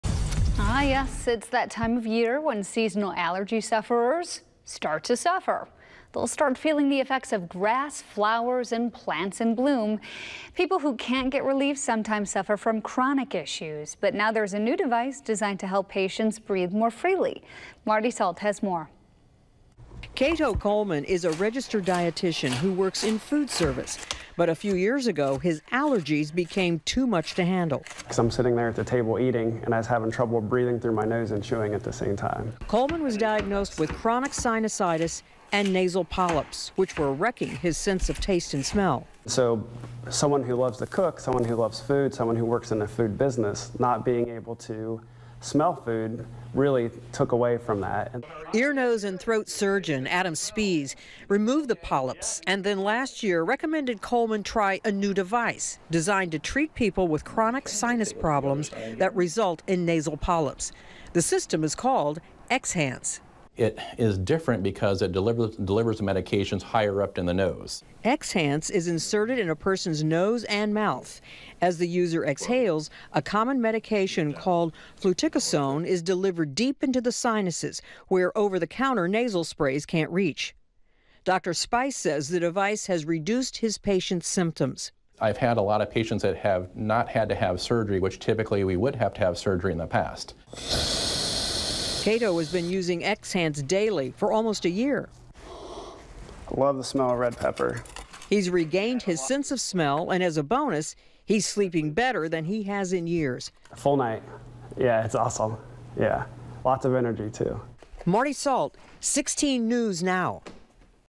Medical Moment: XHANCE gives patient his sense of smell back
There’s a new device designed to help patients who have sinusitis and nasal polyps.